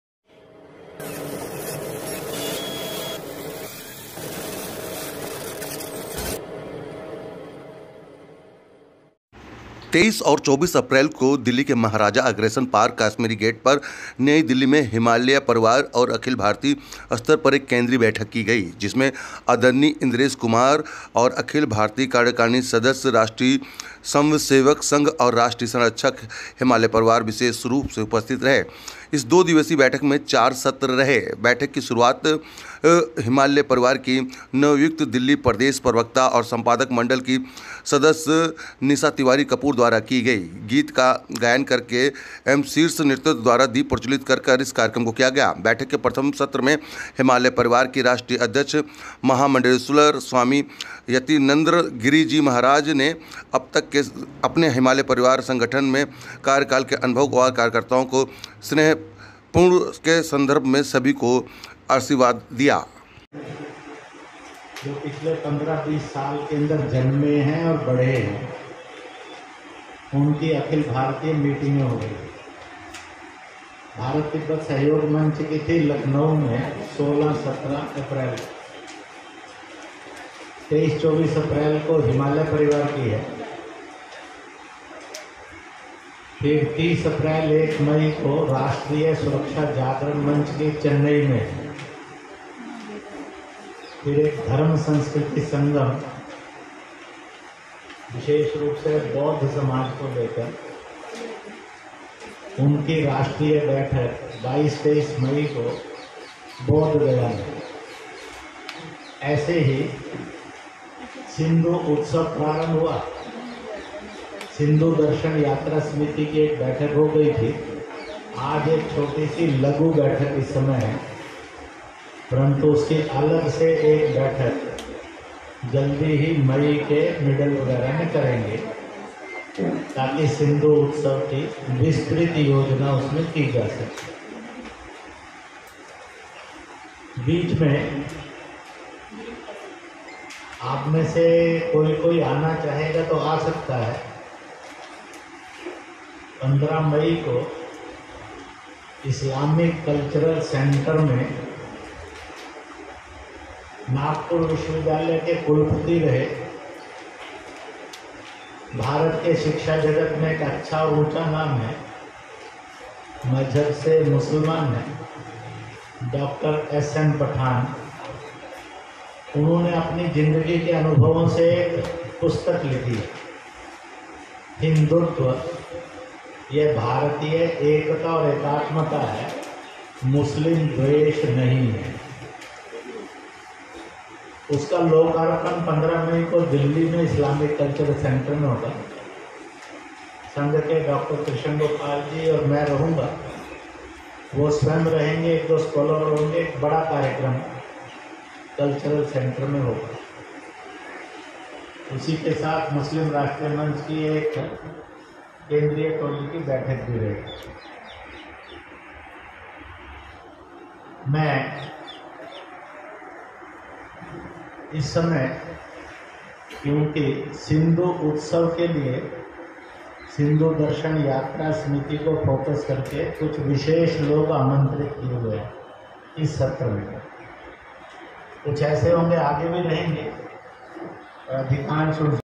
हिमालय परिवार की 2 दिन की दिल्ली में हुई बैठक में लिए गए कई अहम निर्णय
Report By :- Neeraj Awasthi

Camera By :- Manoj Kumar